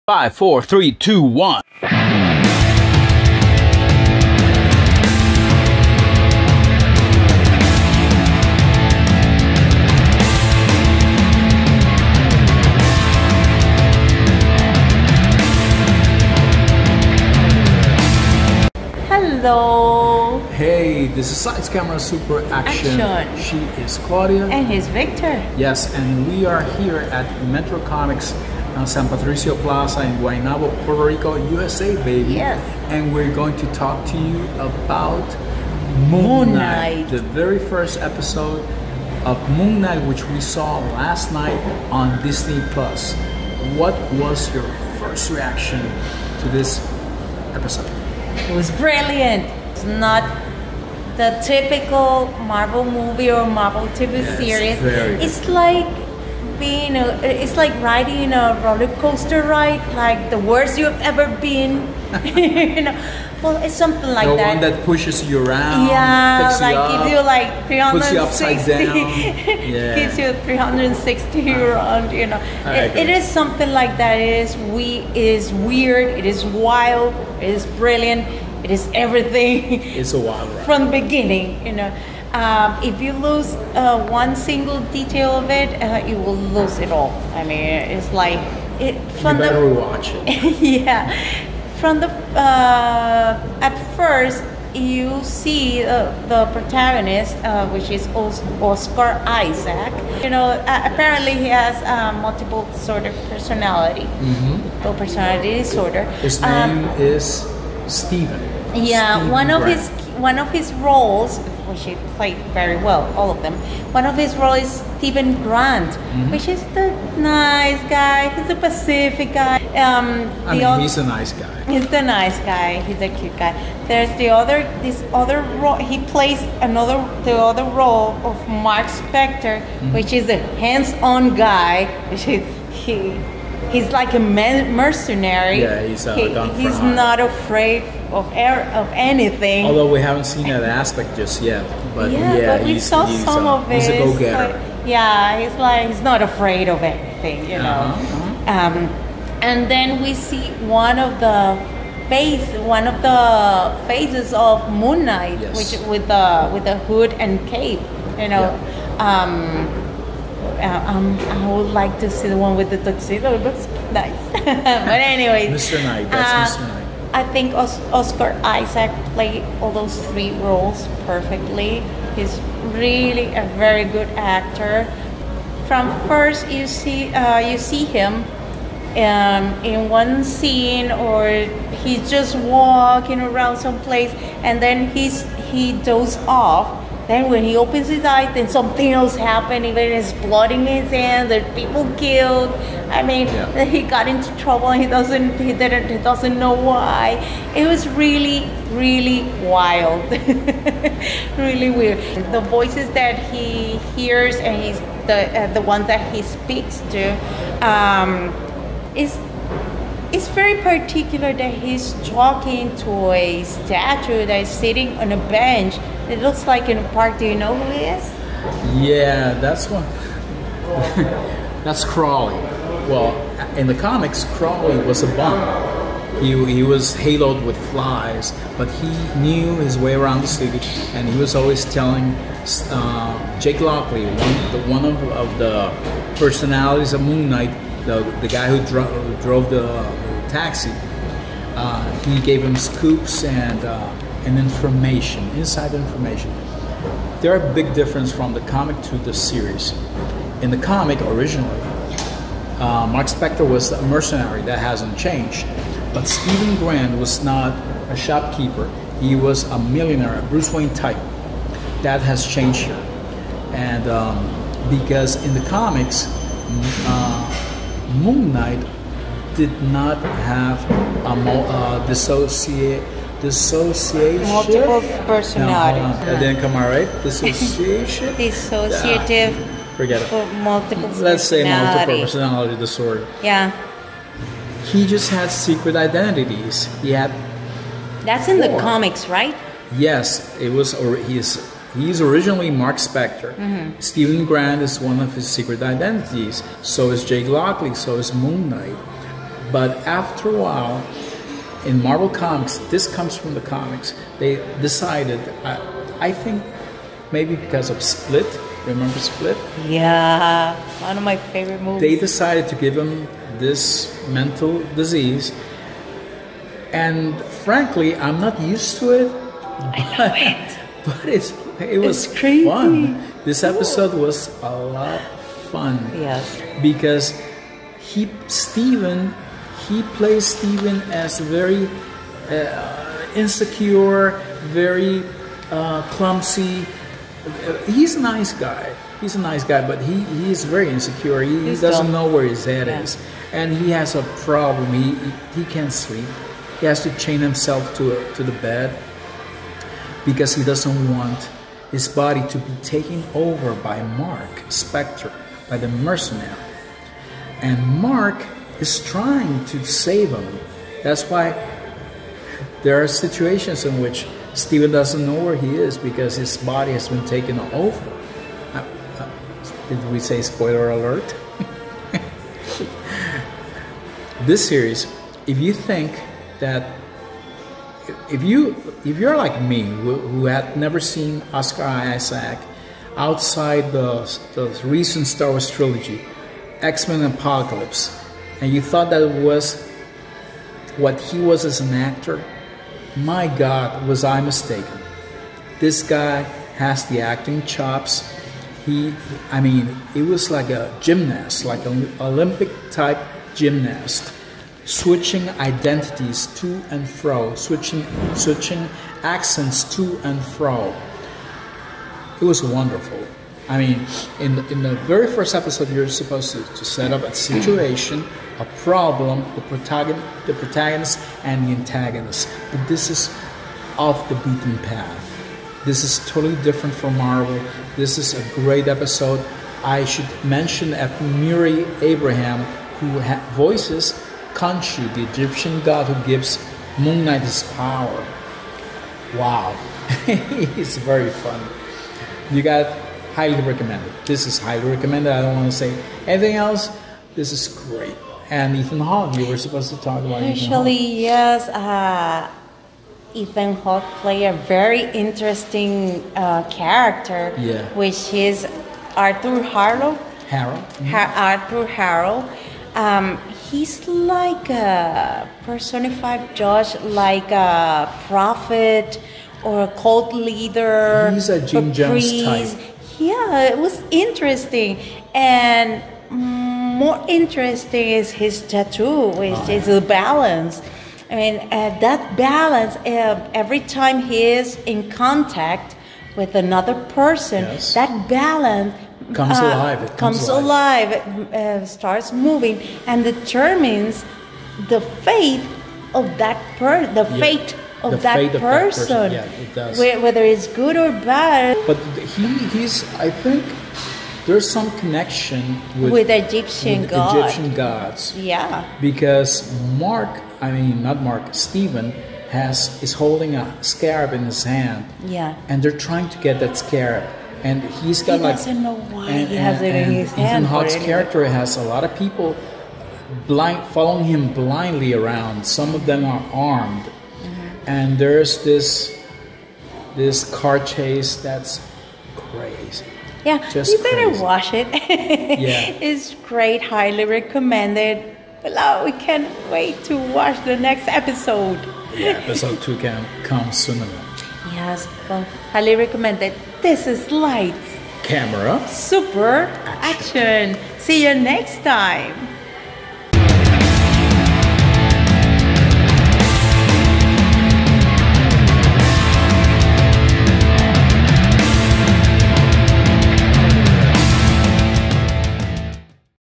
LIGHTS CAMERA SUPER ACTION- Moon Knight Episode 1: Wild! Weird! Bonkers! And we LOVED it!
WE LOVE COMICS AND WE LOVE FILMS

Your favorite Comic-Book, Science Fiction And Fantasy Movie News Show!